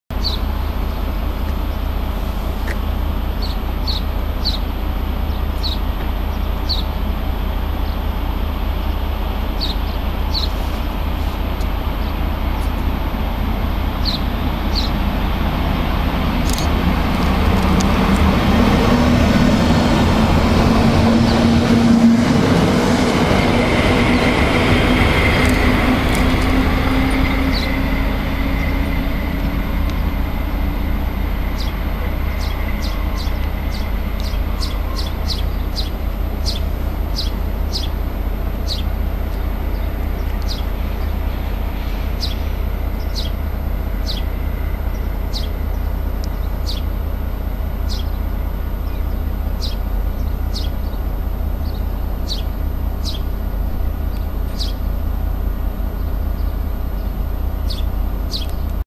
112 166 im Bahnhof Saalfeld (Saale)
112 166 verlässt mit dem Wittenberger Steuerwagen der WEE (Weser Ems Eisenbahn) als RB25 mit ziel Halle/Saale Hbf den Bahnhof Saalfeld (Saale)